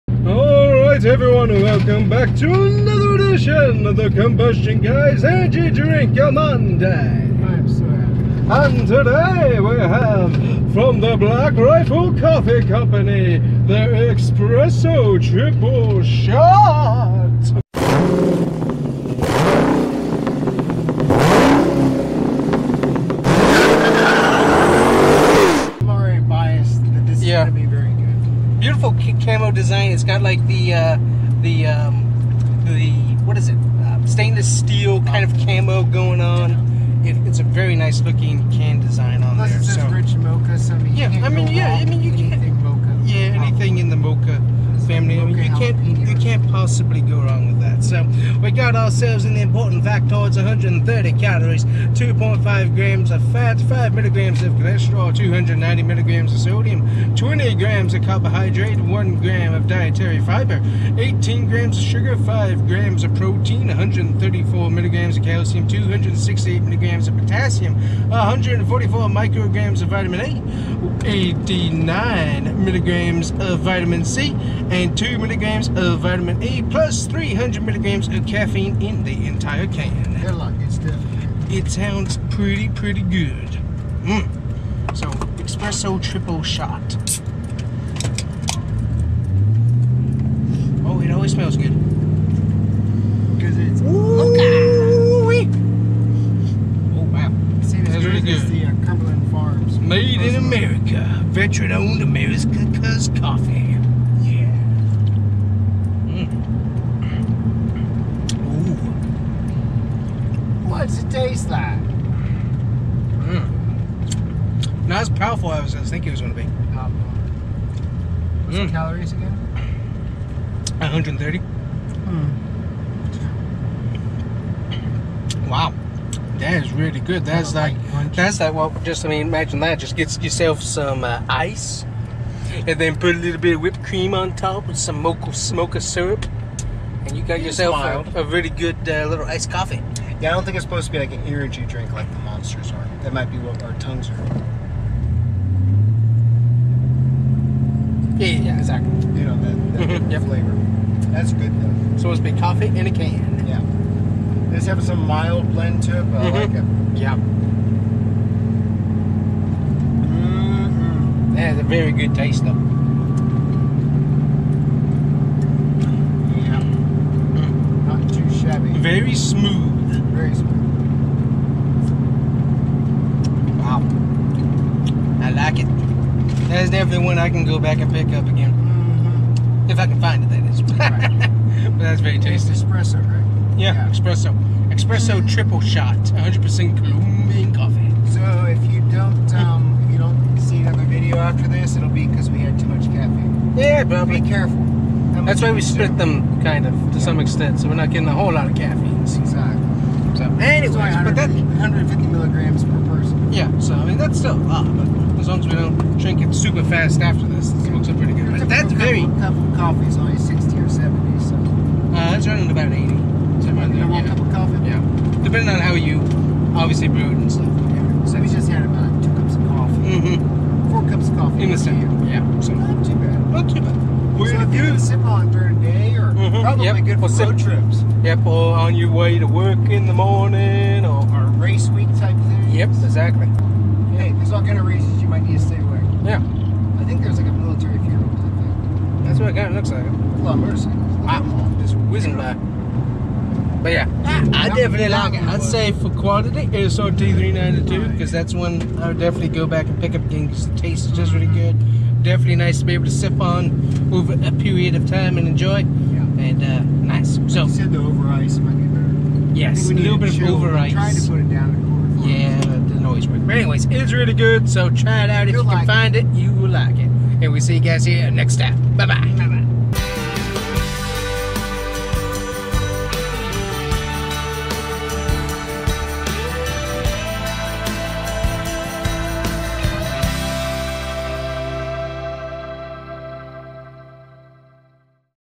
TRYING BLACK RIFLE COFFEE COMPANY ESPRESSO TRIPLE SHOT ENERGY COFFEE MONDAY
Today we're trying Black Rifle Coffee Company Espresso Triple Shot!
Energy Drink Review SRT 392!
Energy Drink Monday

Check Bear out at The Combustion Gamer Youtube Channel!

Check out the Pace Car Podcast!